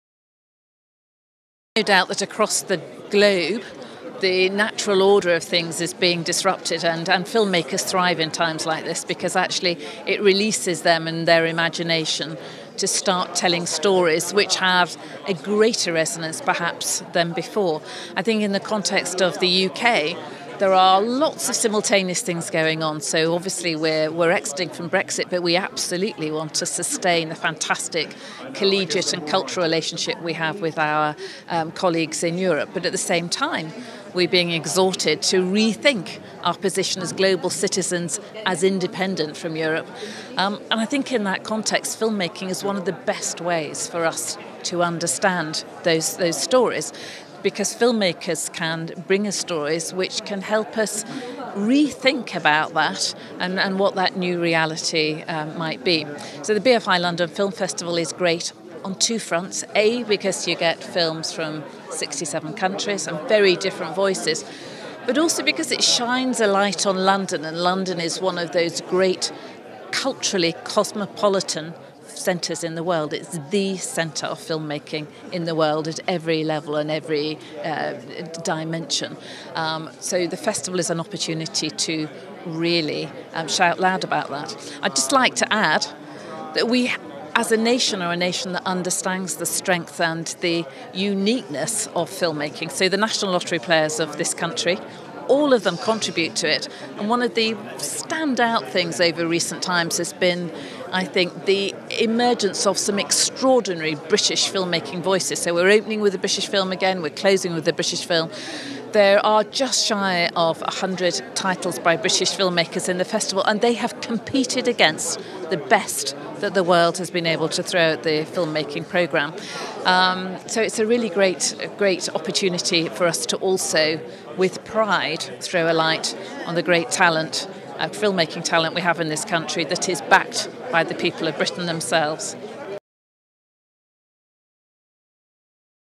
BFI CEO Amanda Nevill Interview British Film Industry & Brexit
Red Carpet news TV talks to Directors and filmmakers at the launch of the BFI London Film Festival 2017 which takes place from the 4th to 15th October. Films from the line-up attending the launch include Opening Gala Breathe, Ghost Stories, On Chesil Beach, Funny Cow, Tides and Film Stars Don’t Die in Liverpool.